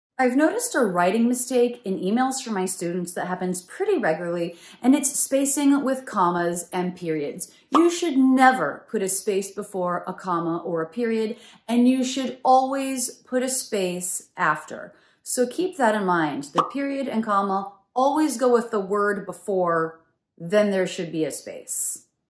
Grammar Mistakes ❌Punctuation
Avoid these grammar mistakes when it comes to spacing with commas and periods in written English.